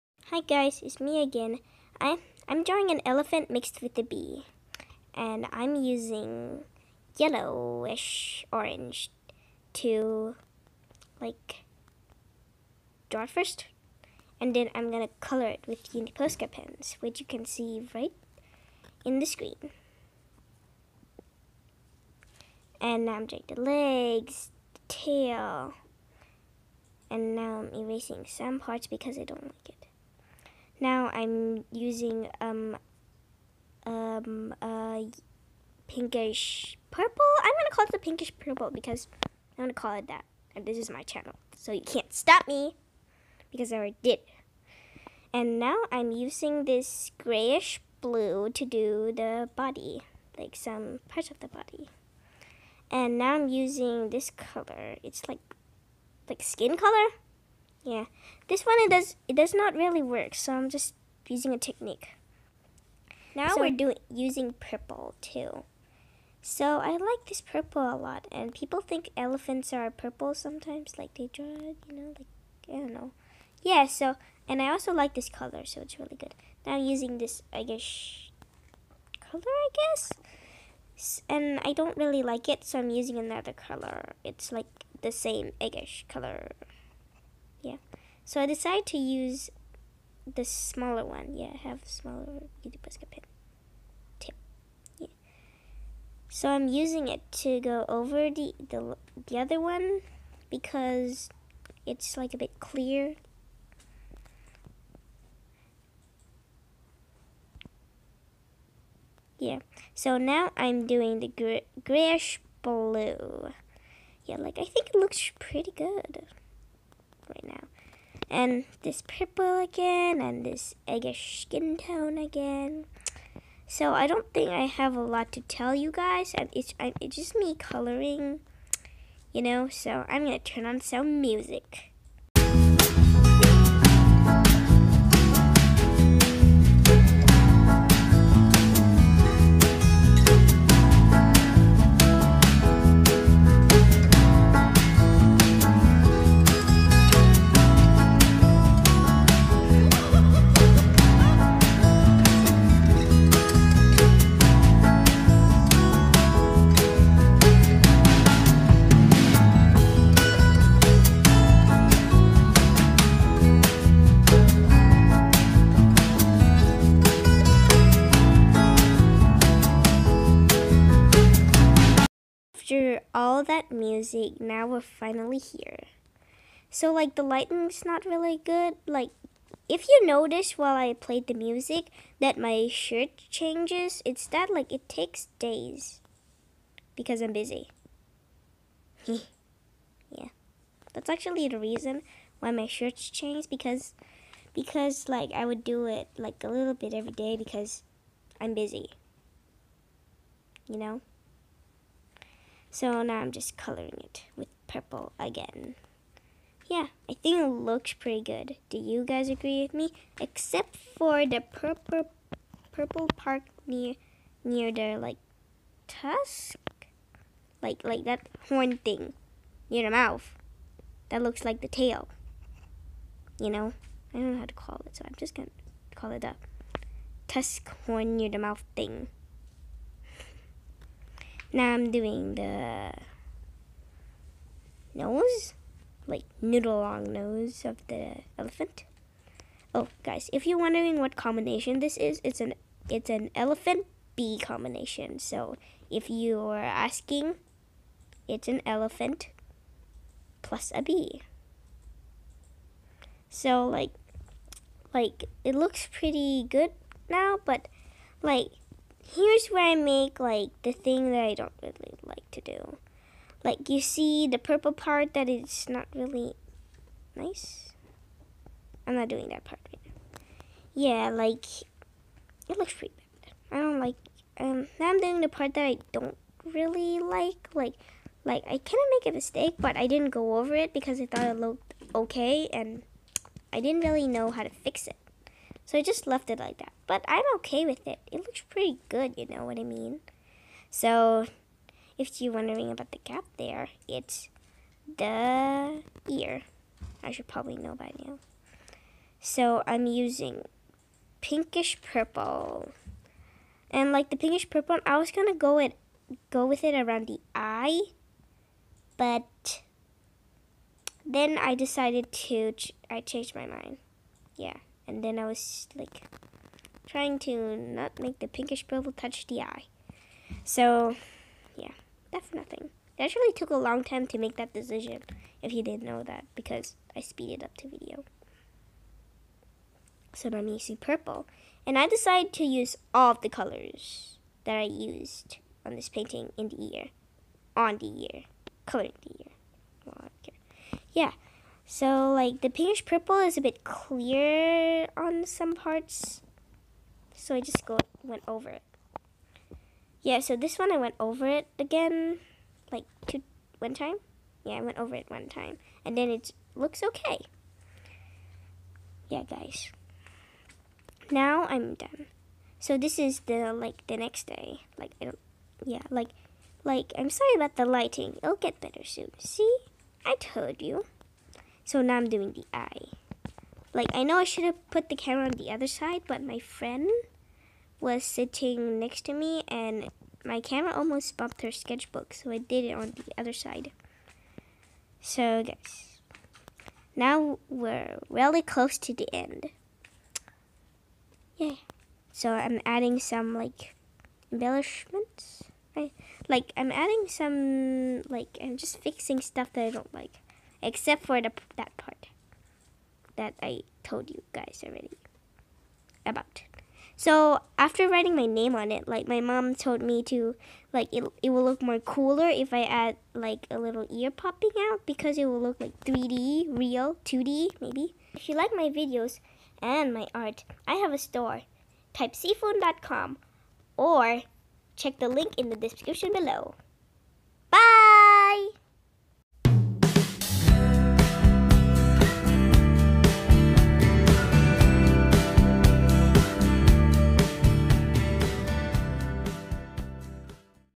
How I use Uni Posca Pen on My painting : Beelaphent ( Bee mix with Elephant ) circle canvas frame
Today I use Uni posca Pen to paint the Beelaphent on a circle canvas frame. Before I paint the  Beelaphent, I already made the background by using acrylic paint.

If you like this piece of my art you can check it out

+ สั่งเสื้อลายภาพวาดสีฝุ่นได้ที่